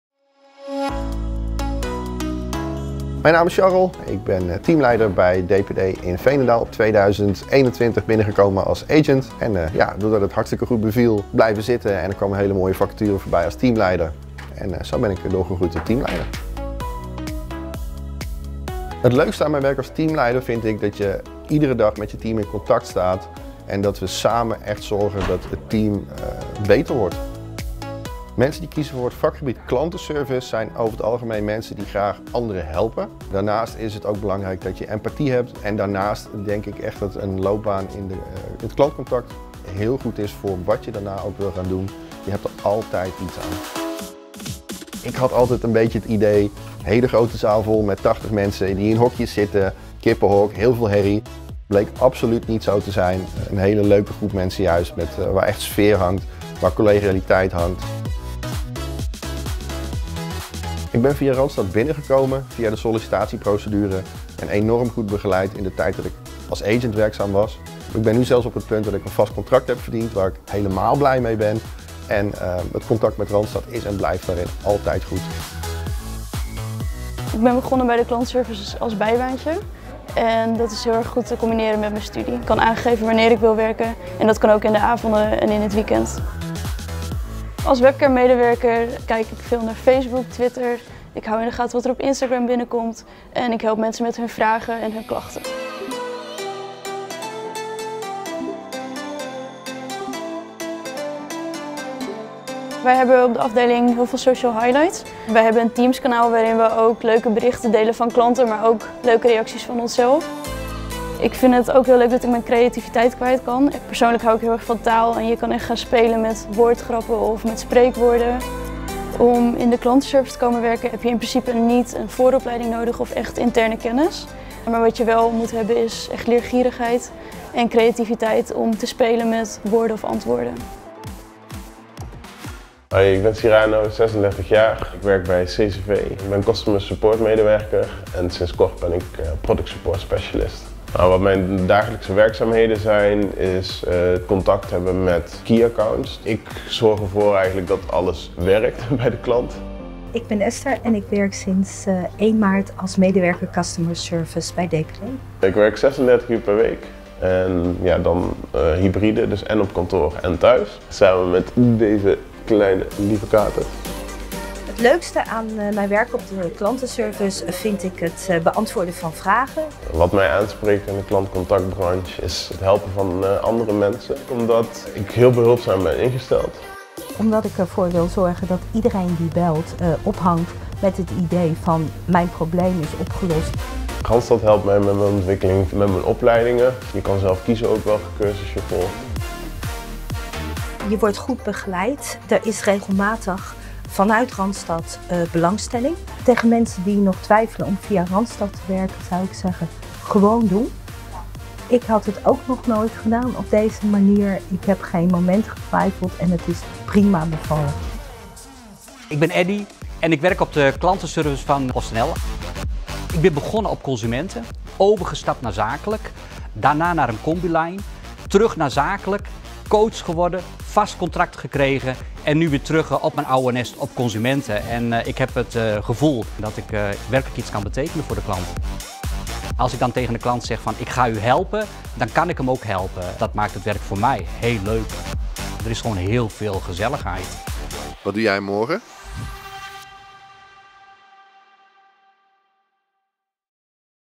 Working in customer service
Working in customer service means helping people and always going for the best possible solution. And did you know that there are a lot of different kind of jobs in customer service?  Probably a lot more than you know right now...Do you want to find out more about working in customer service? Watch this video and find out yourself!

0:19 working together as a team
0:44 customer service clichés debunked
01:00 working via randstad
01:17 the different working hours
01:58 useful skills to have to work in customer service
02:35 working from home
02:44 the most fun things about working in customer service
03:03 how randstad will help you with your development
03:28 career opportunities in customer service